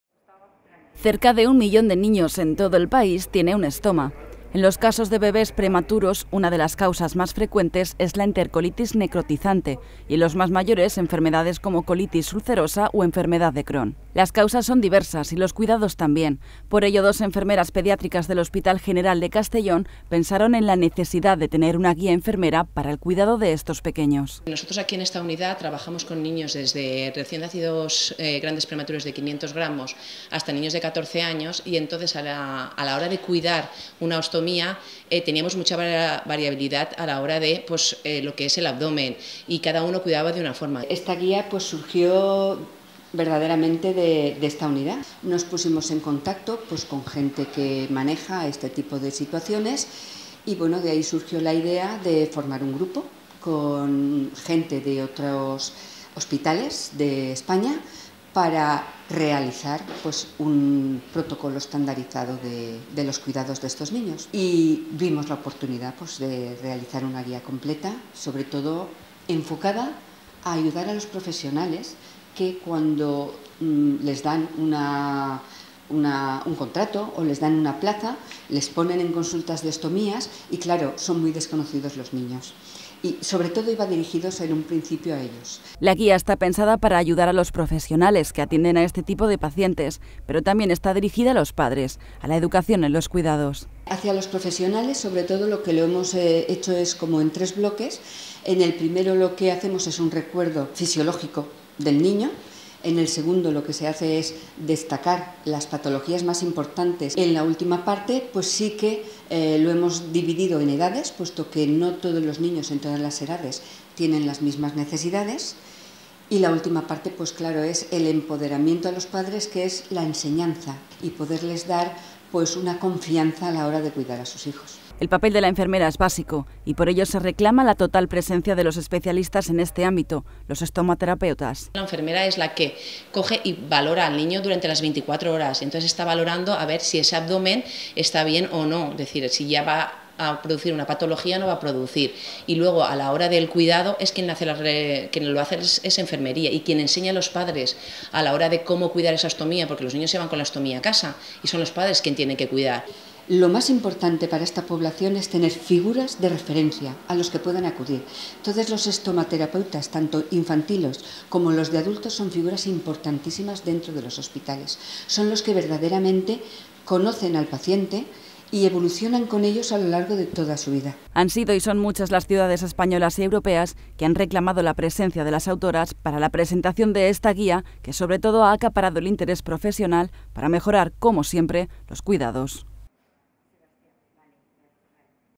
2019, el año que se alcanzó la normalización de la ostomía | Cecova TV
En este repaso de 2019 no podemos olvidarnos de todas las actividades, jornadas o esfuerzos que se han hecho para normalizar esta intervención que hasta hace poco los pacientes ocultaban llegándoles incluso a provocar problemas psicológicos. Gracias a la Enfermería, por sus cuidados y su visibilización, la ostomía deja de ser un tabú. Un ejemplo es la primera guía enfermera en Europa sobre los cuidados tras una ostomía en pacientes pediátricos y que guía a padres, madres y profesionales. Sus autoras, dos profesionales de la salud del Hospital General de Castellón.